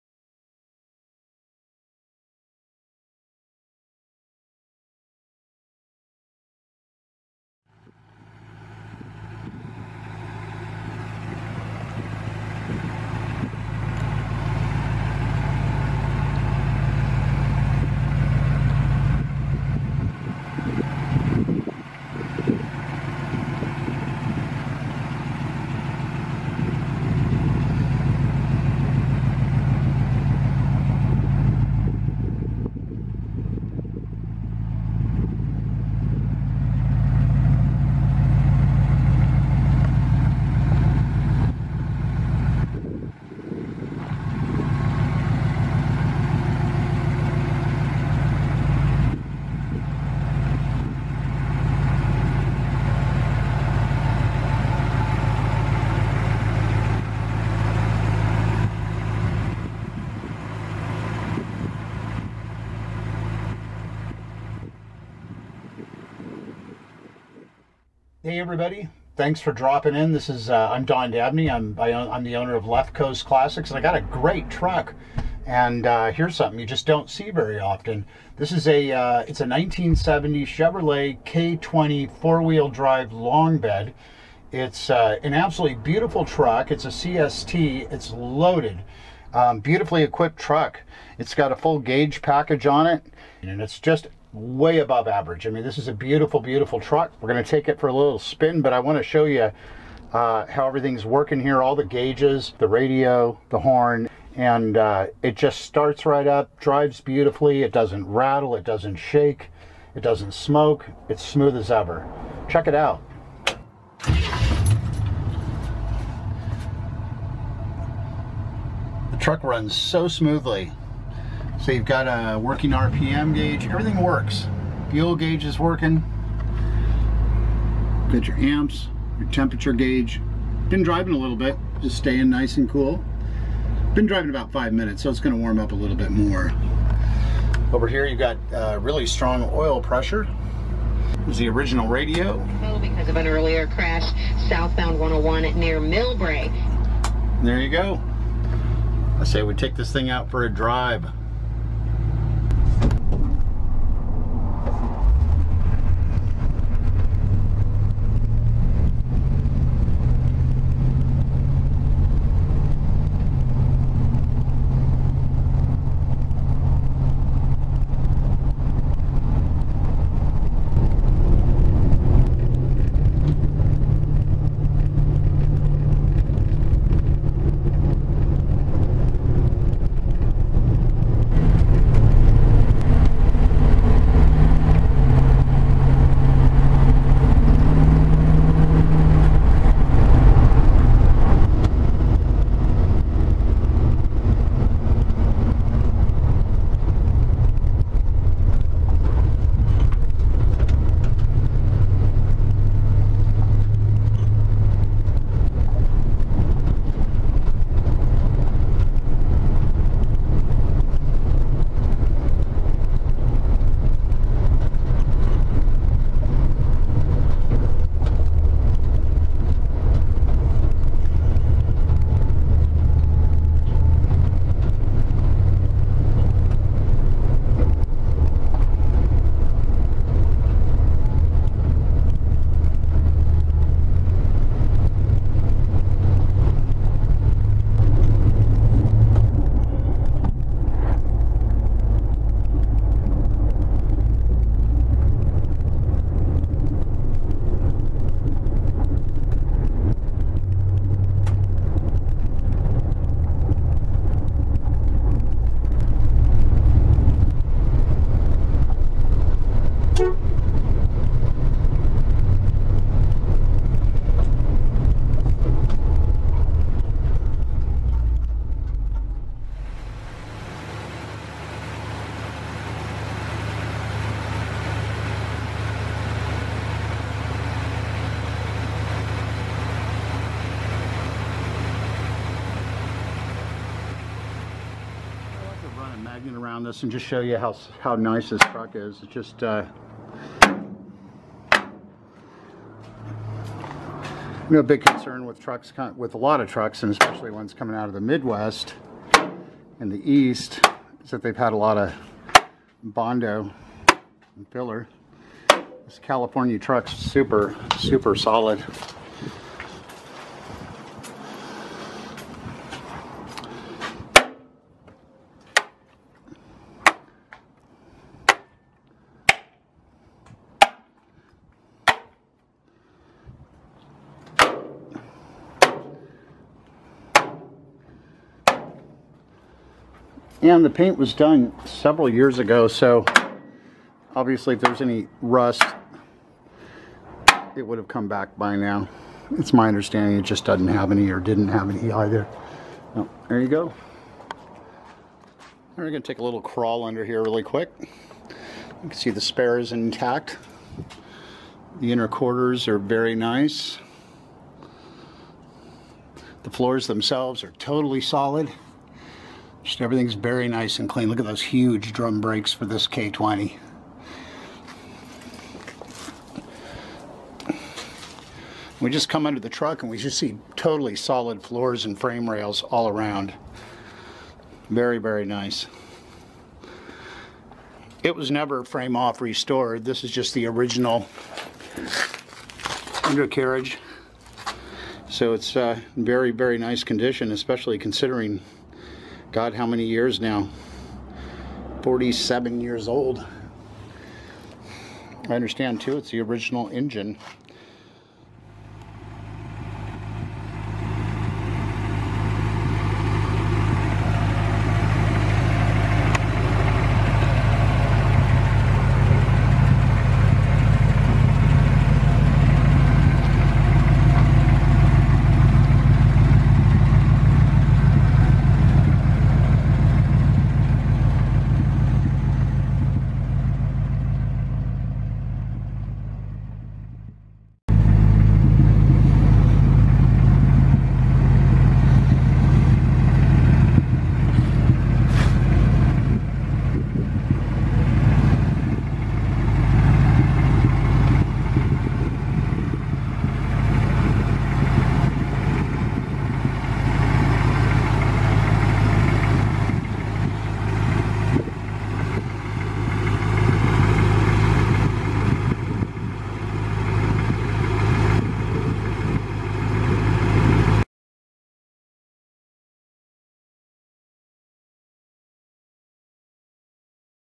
Restored 1970 Chevrolet K20 CST 4x4 Pickup for Sale
to see this 1970 Chevrolet K20 CST 4x4 Pickup for sale from Left Coast Classics in Sonoma California.

This is a truly stunning California truck still bearing it's original-issue black and gold license plates and restored to the 9's. Includes the original owners manual and Protect-o-Plate and many records to go with it.

The exterior of this CST/20 was treated to a high-end repaint several years ago and looks better than factory. Notice the mirror-like reflections in the video included. At that time all new GM Molding, emblems and bezels were installed along with newly upgraded cab and front end mounting bushings, a new GM grille assembly, custom built 16x8 steel wheels with stock back-space, new GM tailgate, new locking fuel doors, gas lines, senders and new mirror heads.

Extremely clean and tidy 1970 Chevrolet K20 CST 4x4 Pickup for sale with just 105k original miles. This classic K20 4x4 was purchased new in May of 1970 at Fitzpatrick Chevrolet in Concord, California and even includes it's original owners manual and Protect-o-Plate. It has been appraised at #2 condition.

The truck starts runs and drives beautifully. It has the original, stock 350ci V8 and the original 350TH transmission and transfer case which were rebuilt with an upgraded torque converter/shift kit. The truck is equipped with 4:10 gears and a Detroit locker. The exhaust, suspension, and mechanicals are all performing well. It has newer brakes, bearings and wheel cylinders. The cooling system was gone through not too many miles ago with new water pump, radiator, hoses, belts, and thermostat. At that time it received a new alternator, rebuilt original Quadrajet carburetor, starter and alternator and motor mounts and more. The truck is also equipped with auxiliary battery and wiring. The truck runs, drives and performs beautifully.

for the full detailed description and to see the photo gallery of this nice Chevy truck - then give me a call with your best offer